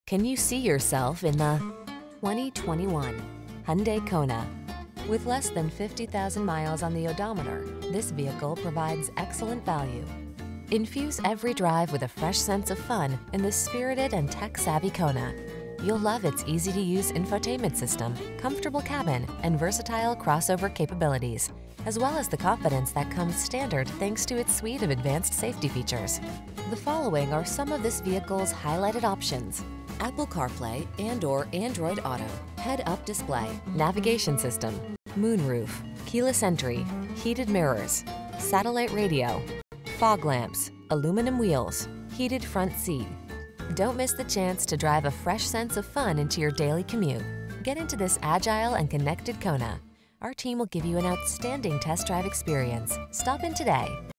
2021 Hyundai Kona Kennesaw, Marietta, Smyrna, Woodstock, Atlanta, GA P7760A
Thunder Gray Used 2021 Hyundai Kona Ultimate available in Kennesaw, Georgia at Town Center Nissan. Servicing the Marietta, Smyrna, Woodstock, Atlanta, GA area.

2310 Barrett Lakes Blvd NW